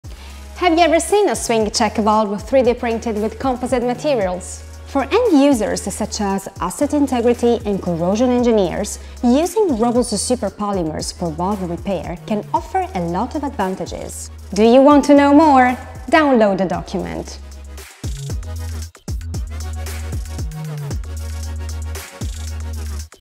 3D Printed Swing Check Valve Trim – Metal Replacement
Roboze Additive Manufacturing can provide metal replacement solutions for swing check valves.
Replacing metal with Roboze high-performance polymers can provide cost and lead time savings. Additive manufacturing can also be used to create complex geometries with high precision, reducing material waste and production time. However, it is crucial to meet ISO, API, and other relevant standards for industrial additive manufacturing to ensure the reliability and safety of the products. Roboze closely works with experienced valve repair services companies with guaranteed quality and warranty, helping customers worldwide maintain optimal valve performance through certified partners.
Traditional methods have their limitations, they are still widely used for swing check valve production, especially in high-volume applications or when specific material properties are required. However, Roboze industrial 3D printing offers an alternative approach that can overcome some of these limitations, allowing for more design flexibility, reduced lead times and lower costs.
For end-users such as asset integrity and corrosion engineers, using Roboze super polymers for swing check valve repairs can provide numerous benefits.